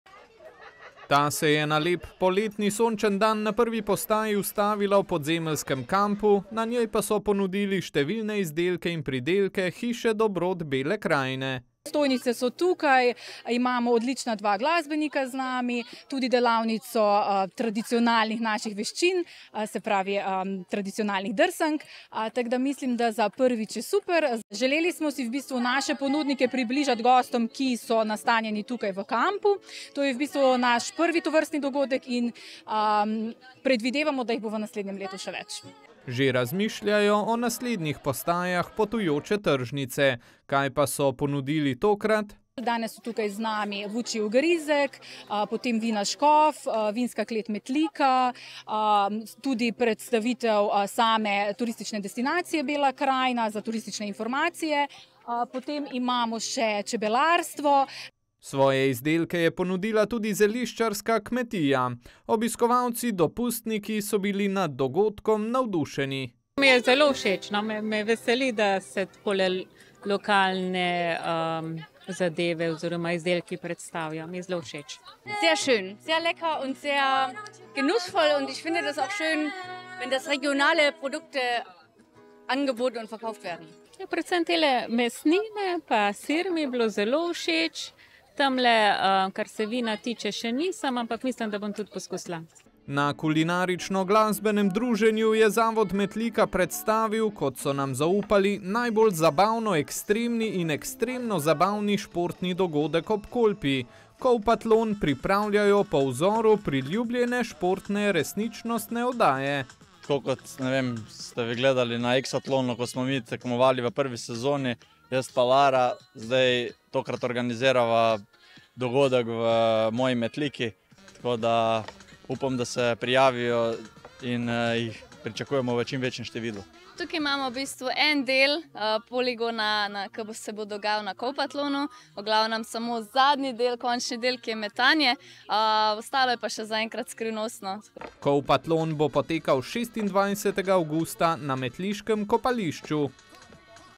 Potujoča tržnica Hiše dobrot Bele krajine
PODZEMELJ - Bi se vam dopadlo, da bi ob vašem dopustovanju pred vašim pragom zbrali vse najboljše lokalne dobrote? Tako so v Zavodu za turizem, kulturo, šport in mladino Metlika prišli do ideje o potujoči tržnici.